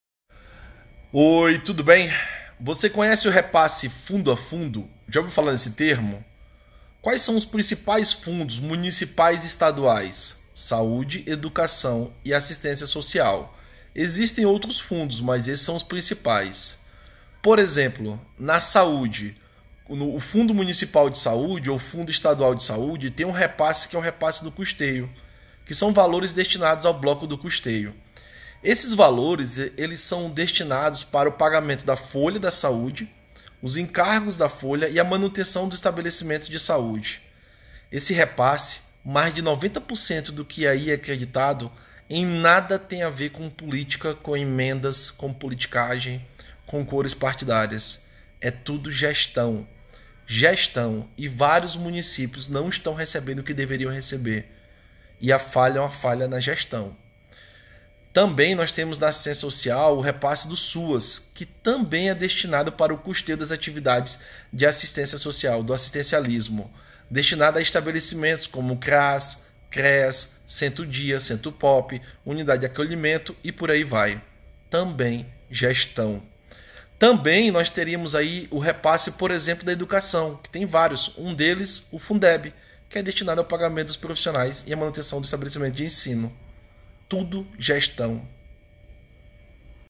Repasse fundo a fundo: É possível captar recursos?
Recurso financeiro tem, o que falta é capacidade técnica.

AJUDO você a trabalhar com captação de recursos em todas as suas fases!

Até o próximo post!